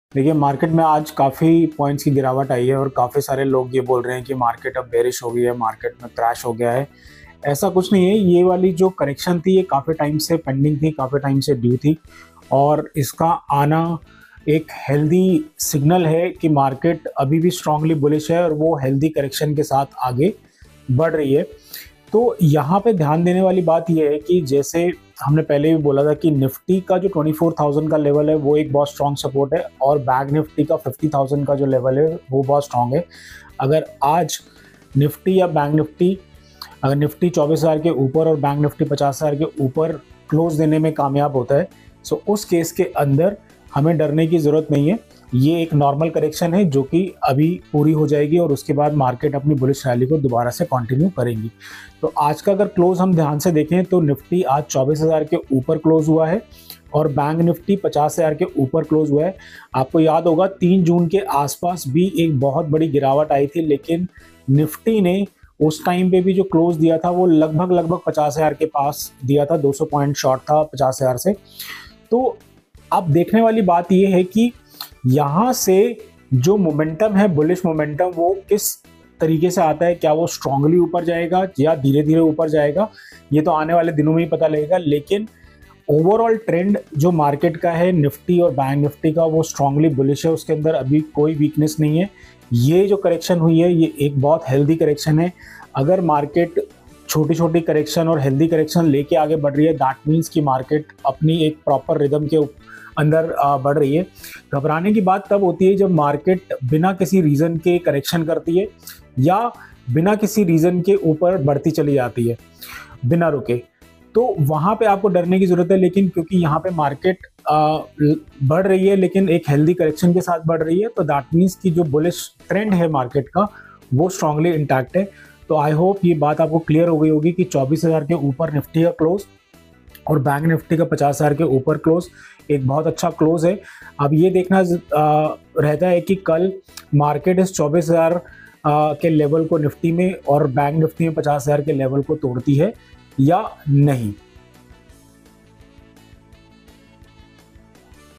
stock market crash today?? watch to understand what it means
Both nifty and banknifty nifty saw a big correction today. Is it stockmarket crash?? We deep dive down to find out what it means